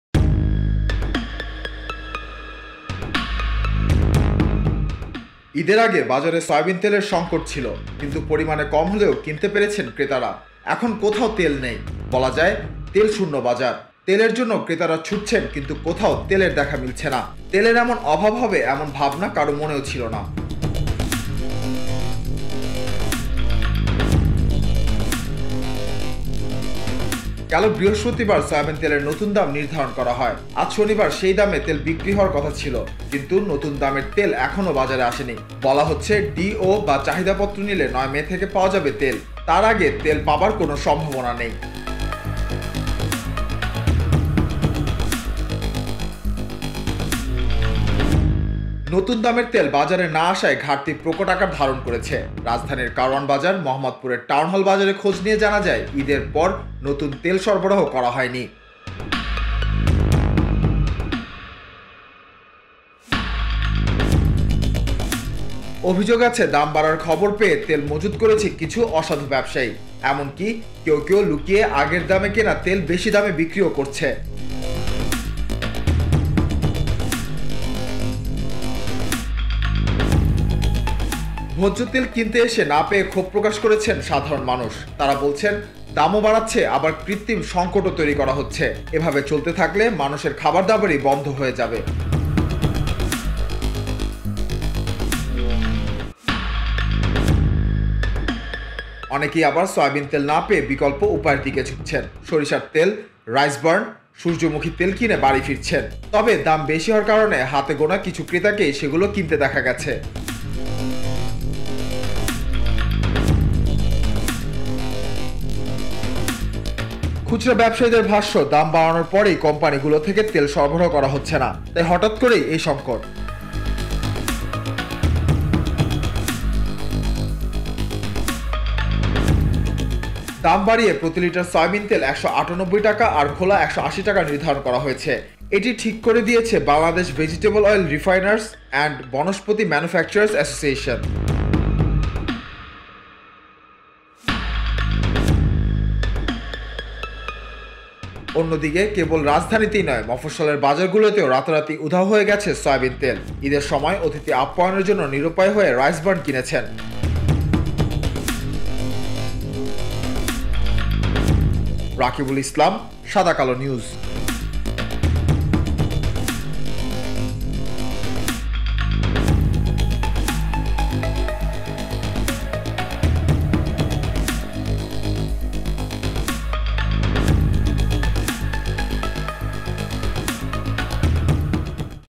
তেলের বাজারে হাহাকার! কোথাও নেই তেল! কবে, কীভাবে মিলবে তা নিয়েও ধোঁয়াশা । Soybean oil । Oil Crisis
ঈদের আগে বাজারে সয়াবিন তেলের সংকট ছিলো। কিন্তু পরিমাণে কম হলেও কিনতে পেরেছেন ক্রেতারা। এখন কোথাও তেল নেই। বলা যায়, তেল শুন্য বাজার। তেলের জন্য ক্রেতারা ছুটছেন কিন্তু কোথাও তেলের দেখা মিলছে না। তেলের এমন আকাল হবে এমন ভাবনা কারও মনেও ছিলো না।